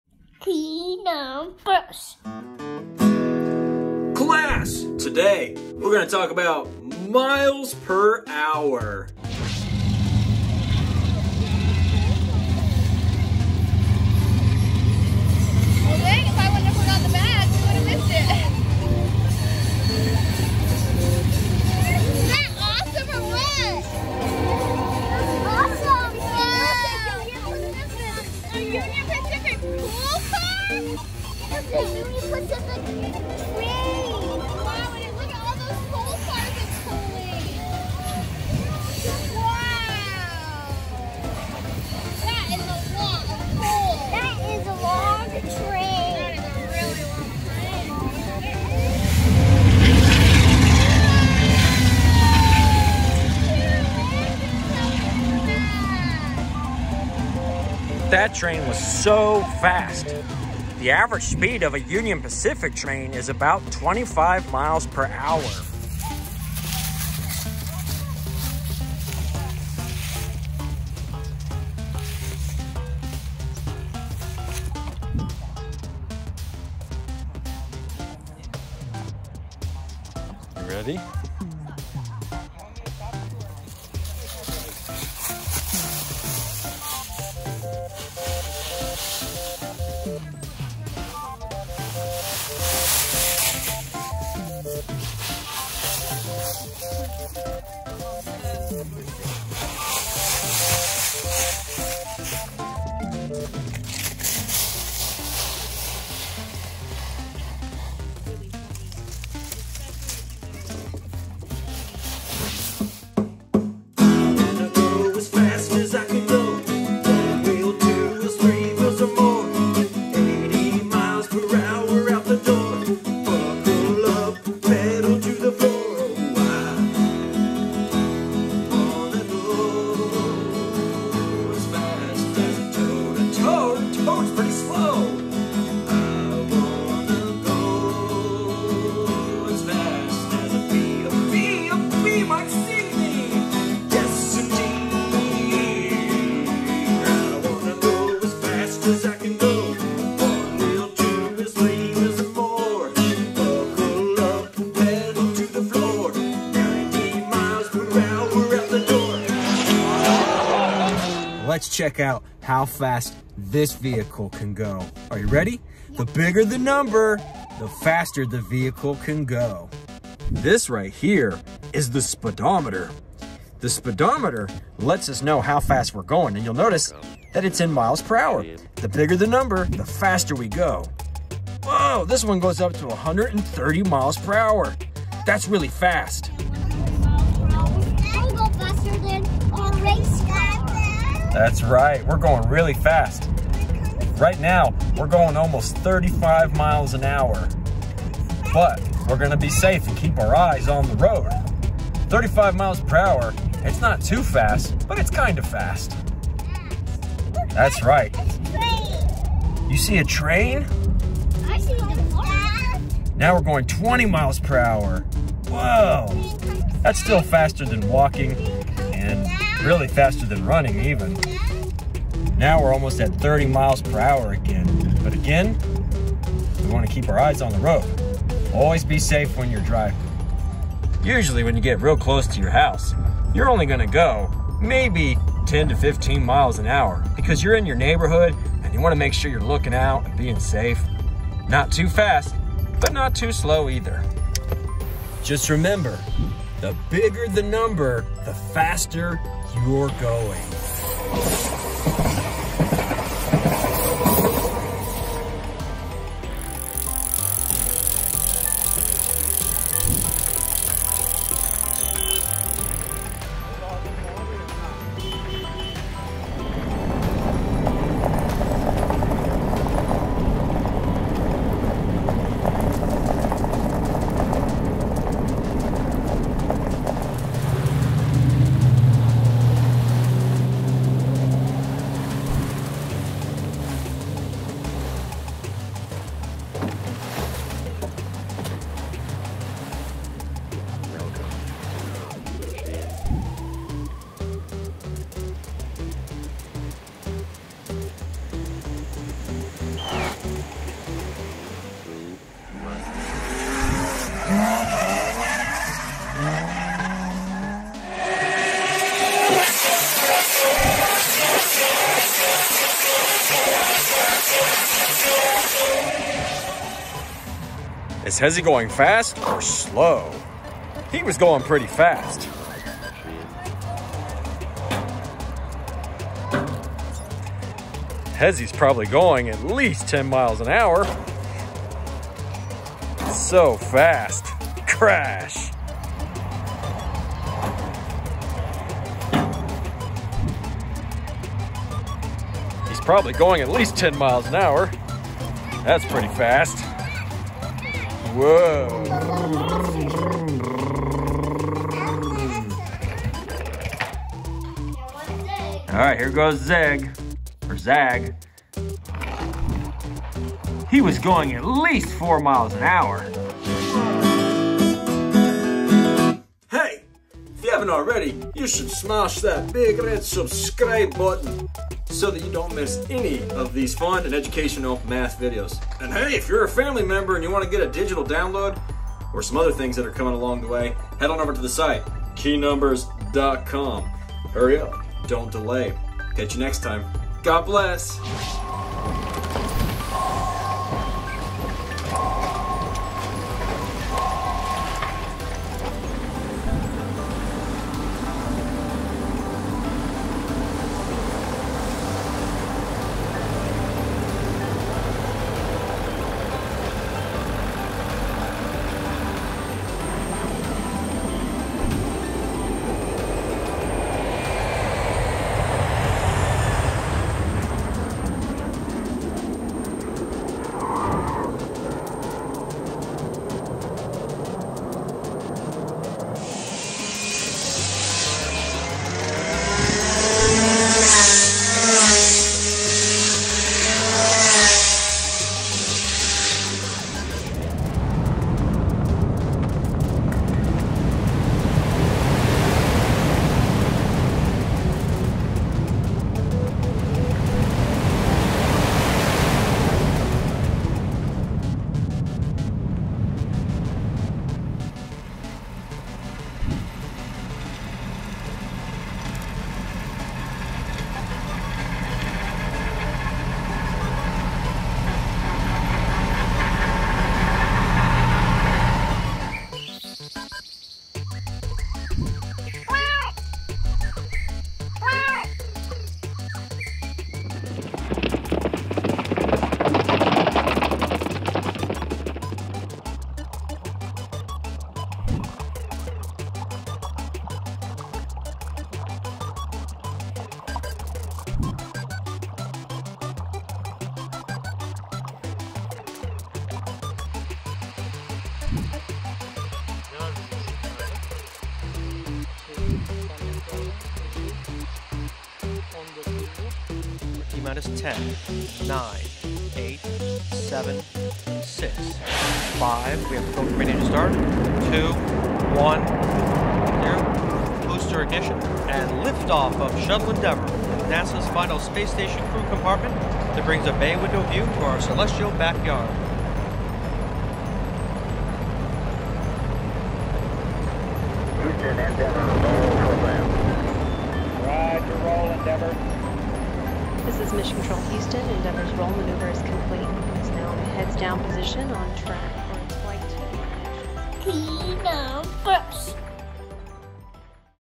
Miles Per Hour: Teach Your Kiddo About Speed | 1st Grade & 2nd Grade Math
Hey class! Have you ever been in the car and thought oh, wow! That's really fast! We're going to look at a special kind of rate, one that tells us how fast or how slow we are going! We're going to talk about miles per hour or mph, in this zany fun math video! Buckle up!